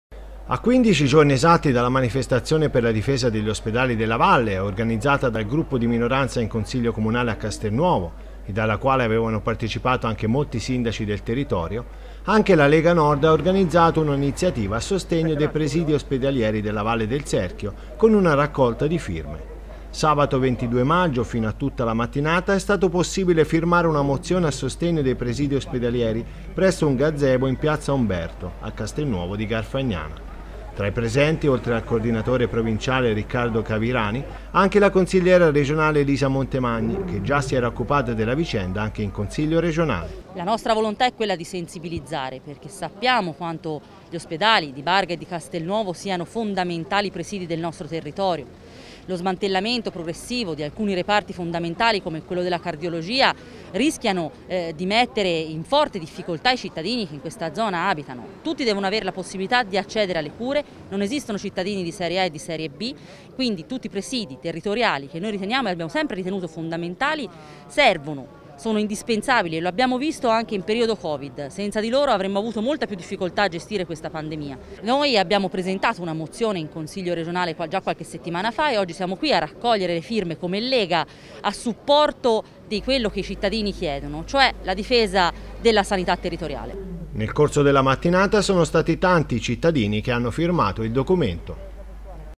Ospedali della Valle, la Lega Nord lancia una raccolta firme a sostegno dei nosocomi
Dopo la manifestazione organizzata dalla minoranza in consiglio comunale a Castelnuovo Garfagnana, anche la Lega Nord si mobilita in sostegno degli ospedali della Valle del Serchio lanciando una raccolta firme.

NoiTv